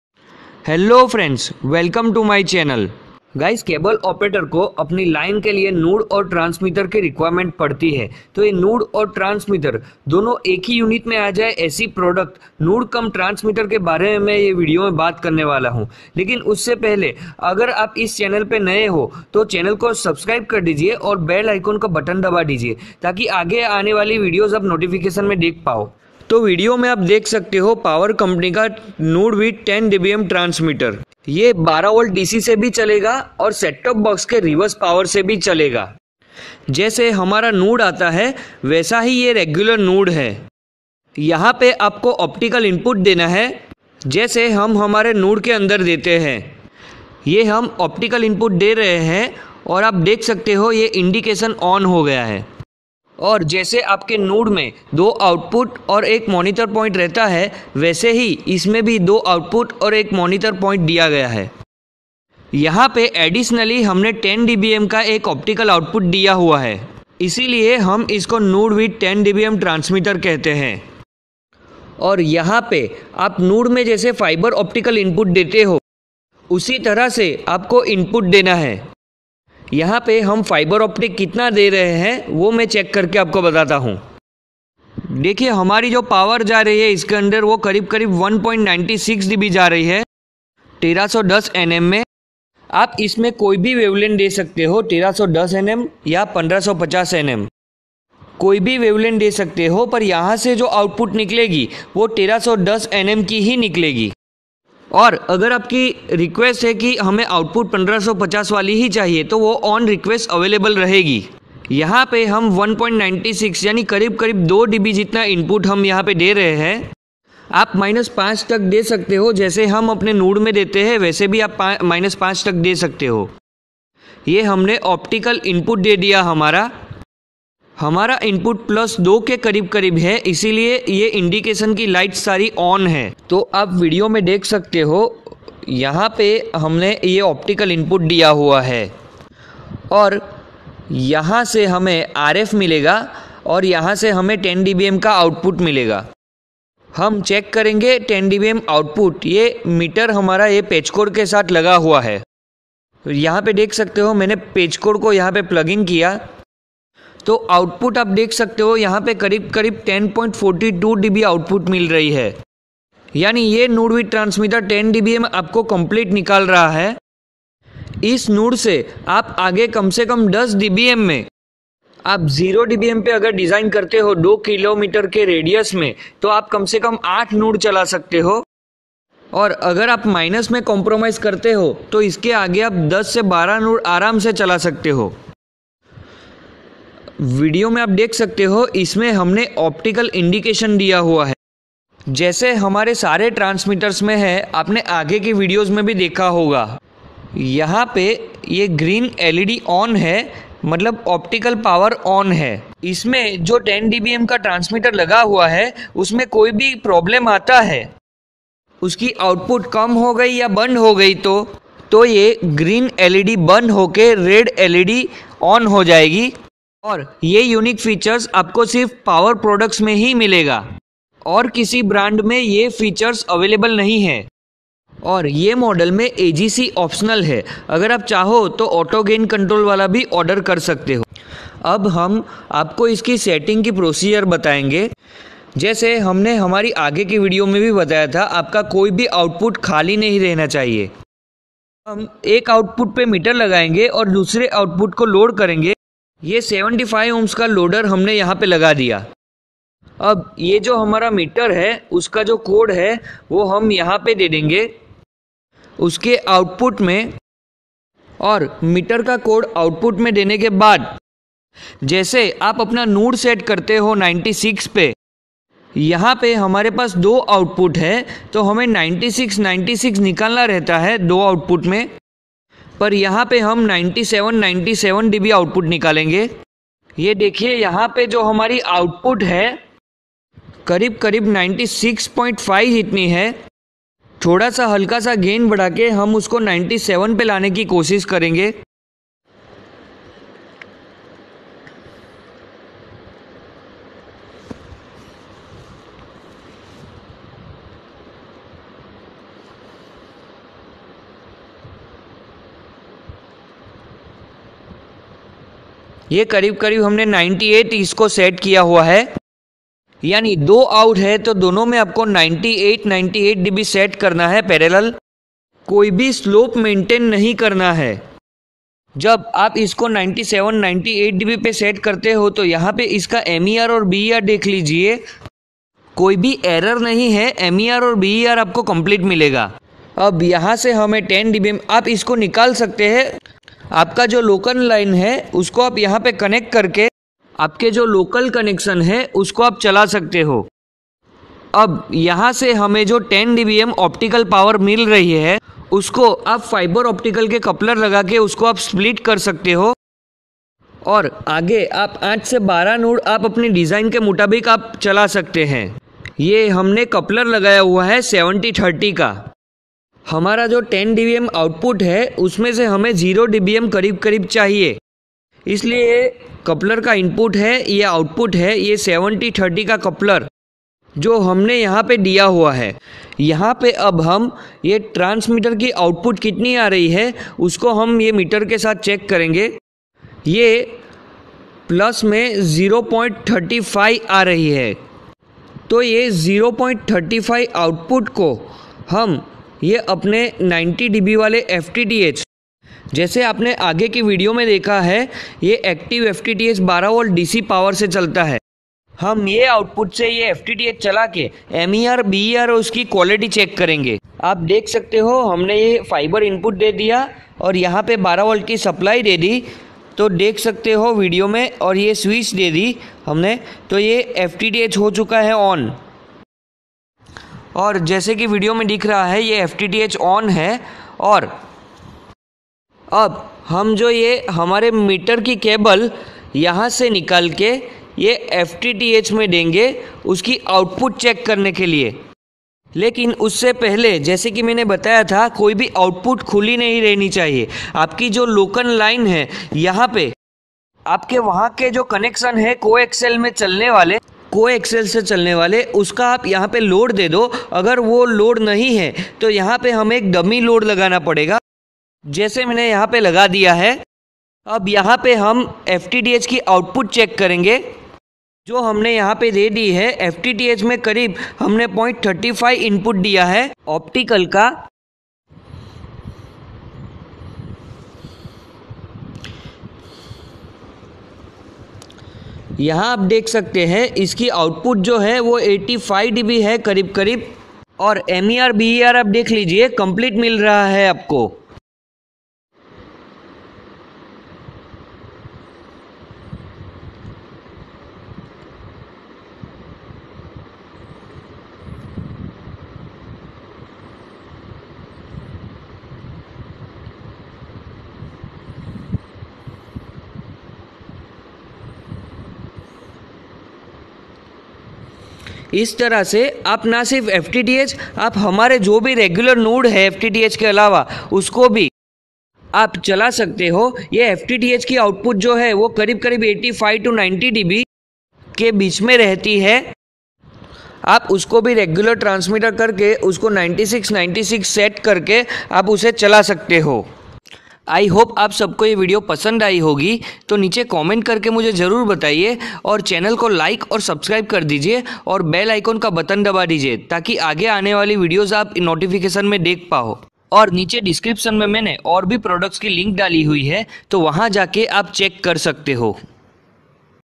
Node With 10 dBm DC Transmitter by Arjun Electronics 2020
Node With 10 dBm DC Transmitter features

- Hybrid Optical Receiver Module Technology

- Excellent Frequency Response

- Optical power Indication

- AGC on request (AGC from +2 to -8 dBm)

- 12 Volt DC Adapter Supply or STB Return Supply

- Digital Display Input

- Output Optical Power Indication

- Variable Gain and Slope Control

Node cum transmitter (& AGC optional) for franchise cantrol room,it is use full for franchise cable operators getting OFC connection  from MSO,it has two RF output ports.with the help of these cable operator can run to RF trunk cables.it also incorporates one DFB  laser which gives an optic output of 10 dBm. cable operator has the facility of simultaneously getting to high qualities RF output and one optic output of 10 dBm. it is as good as making a mini control room at customers premises.

click the link bellow for other products